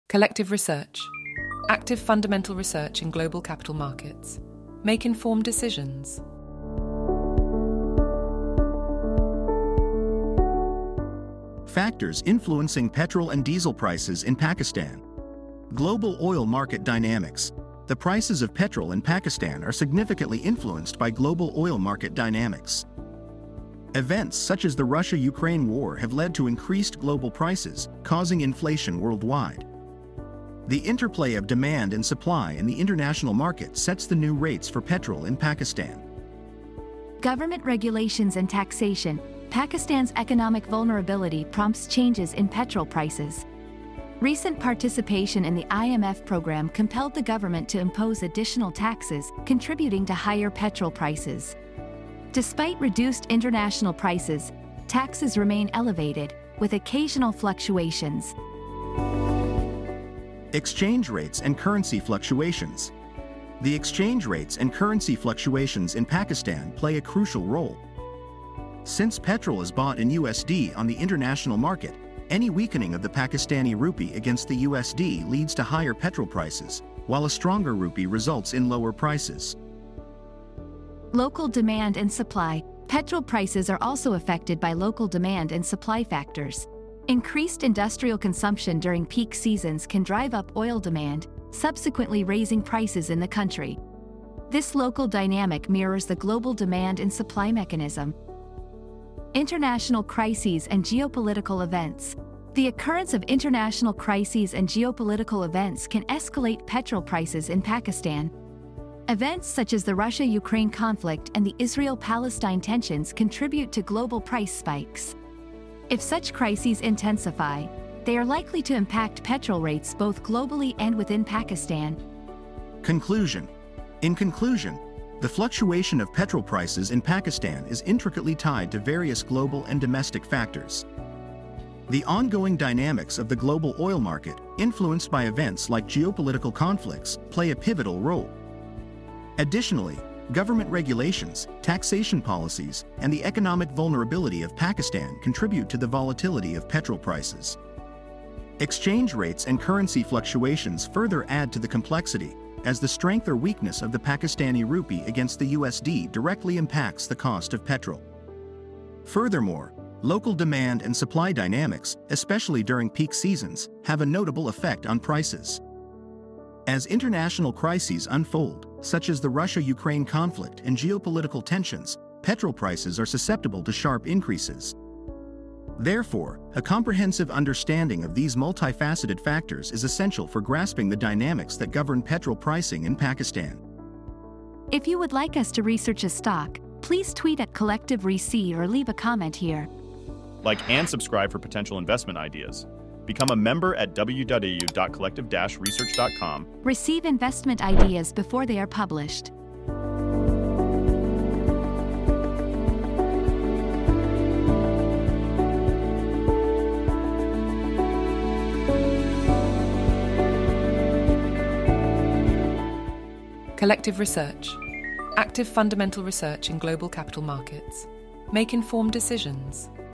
Decoding Pakistan's Petrol Prices: Global Influences, Government Policies, and Local Dynamics!
Explore the intricate web of factors influencing petrol prices in Pakistan with our in-depth analysis. From the global oil market dynamics shaped by events like the Russia-Ukraine conflict to the impact of government regulations and taxation policies, this video delves into the complexities driving fluctuations. Learn how exchange rates and currency fluctuations play a crucial role, and discover the local demand and supply dynamics that contribute to price variations during peak seasons. Stay informed about international crises and geopolitical events that have a direct bearing on petrol rates in Pakistan. Join us in deciphering the comprehensive landscape of petrol pricing – your guide to understanding the forces at play!